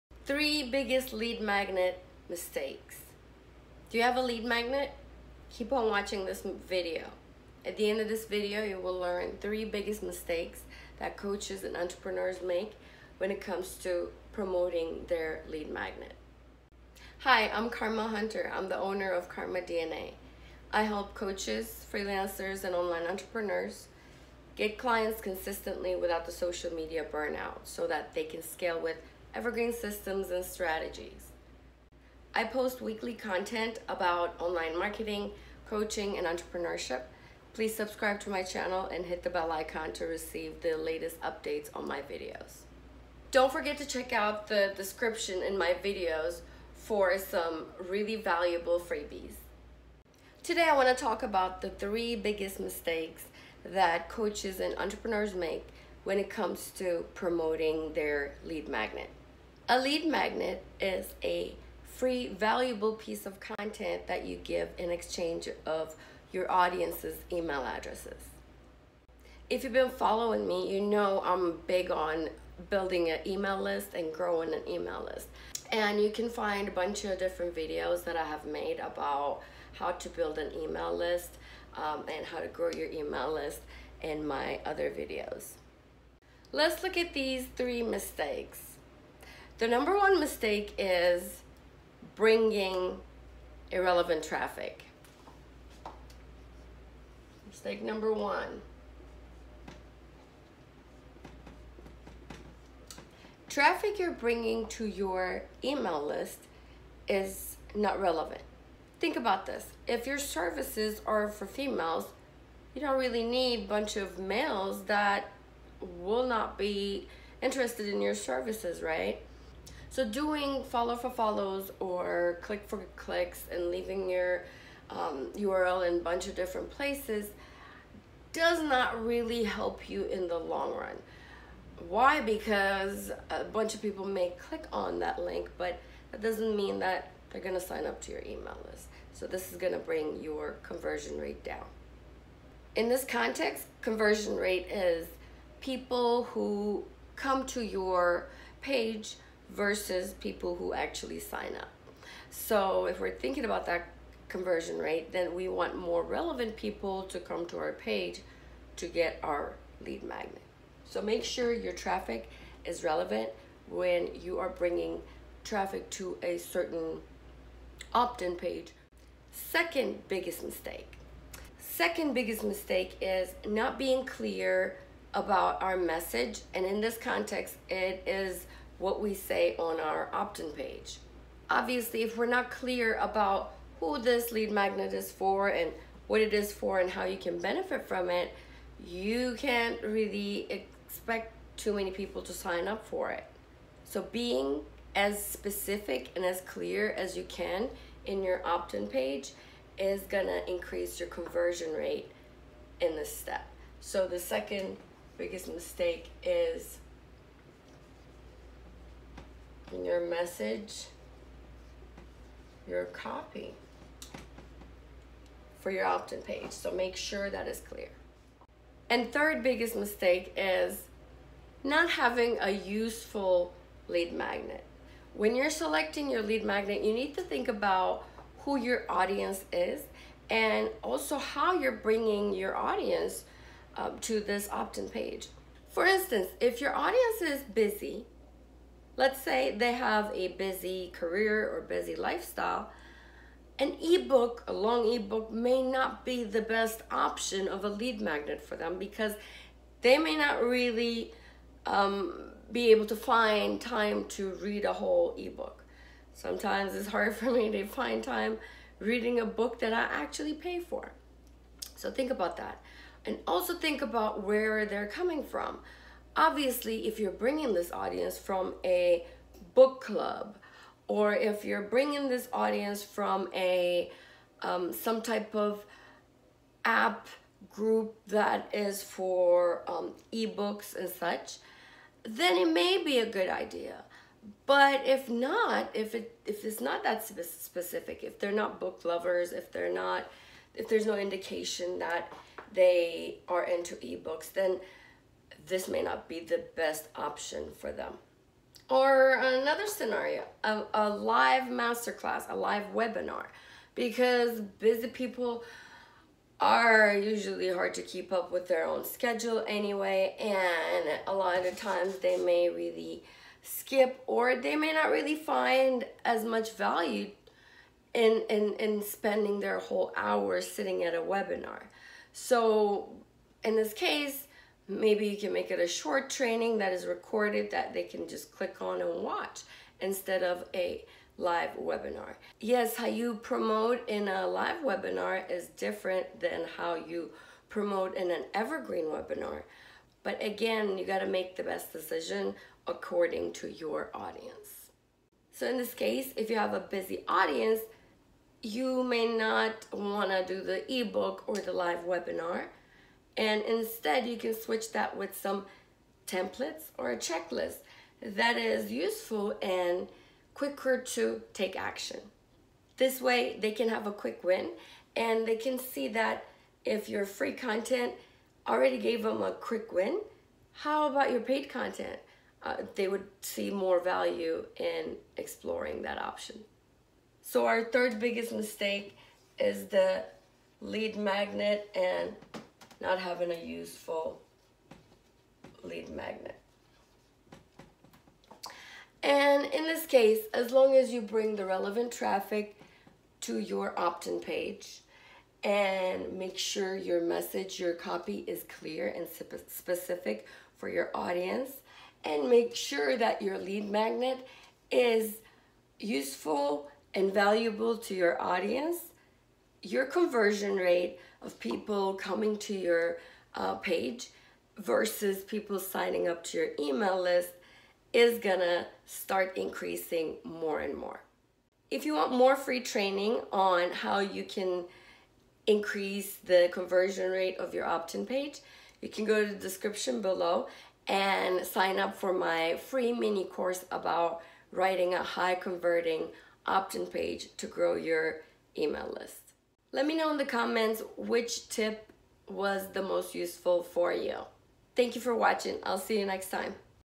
3 BIGGEST Lead Magnet Mistakes that Coaches make
3 BIGGEST Lead Magnet Mistakes that Coaches make

In today's video "3 BIGGEST Lead Magnet Mistakes that Coaches make " I will share with you the 3 BIGGEST Lead Magnet Mistakes that Coaches make that you can avoid, in order to improve your conversion rate.

Sign up to get my FREE Mini Course
Write a High Converting Opt-in Page Guaranteed to Grow Your Email List: